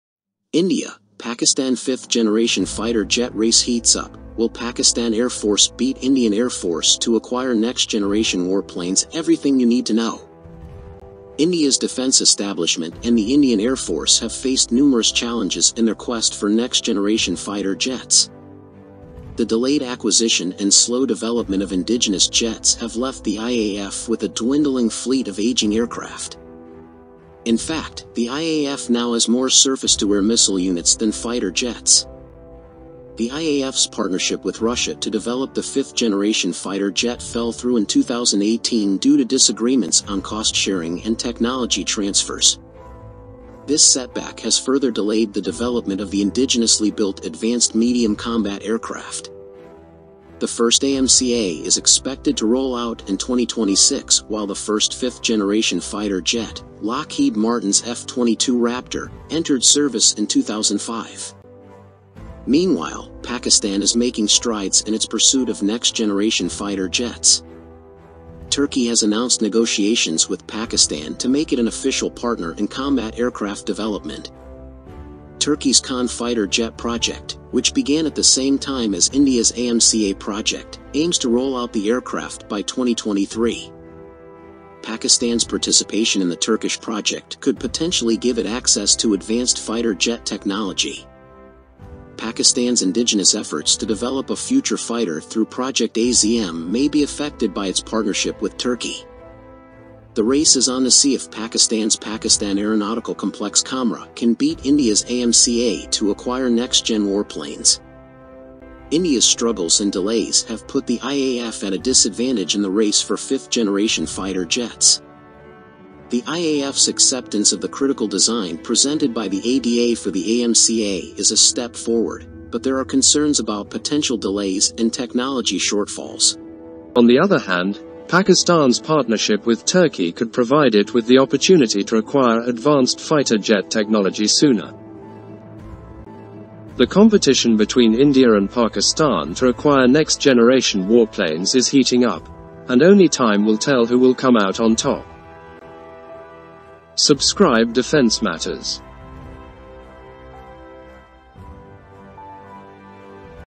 |Inside India's AMCA| |The Next Gen Fighter Jet Race Heats Up!|
India, Pakistan 5th-Gen Fighter Jet Race Heats-Up; Will PAF Beat IAF To Acquire Next-Gen Warplanes Everything you need to know

India's defense establishment and the Indian Air Force (IAF) have faced numerous challenges in their quest for next-generation fighter jets.

The delayed acquisition and slow development of indigenous jets have left the IAF with a dwindling fleet of aging aircraft.

In fact, the IAF now has more surface-to-air missile units than fighter jets.

The IAF's partnership with Russia to develop the Fifth Generation Fighter Jet (FGFA) fell through in 2018 due to disagreements on cost-sharing and technology transfers.

This setback has further delayed the development of the indigenously-built Advanced Medium Combat Aircraft (AMCA).

The first AMCA is expected to roll out in 2026 while the first 5th generation fighter jet, Lockheed Martin's F-22 Raptor, entered service in 2005

Meanwhile, Pakistan is making strides in its pursuit of next-generation fighter jets.

Turkey has announced negotiations with Pakistan to make it an official partner in combat aircraft development.

Turkey's Kaan fighter jet project, which began at the same time as India's AMCA project, aims to roll out the aircraft by 2023

Pakistan's participation in the Turkish project could potentially give it access to advanced fighter jet technology.

Pakistan's indigenous efforts to develop a future fighter through Project AZM may be affected by its partnership with Turkey.

The race is on to see if Pakistan's Pakistan Aeronautical Complex (PAC) Kamra can beat India's AMCA to acquire next-gen warplanes.

India's struggles and delays have put the IAF at a disadvantage in the race for 5th generation fighter jets.

The IAF's acceptance of the critical design presented by the ADA for the AMCA is a step forward, but there are concerns about potential delays and technology shortfalls.

On the other hand, Pakistan's partnership with Turkey could provide it with the opportunity to acquire advanced fighter jet technology sooner.

The competition between India and Pakistan to acquire next-gen warplanes is heating up, and only time will tell who will come out on top.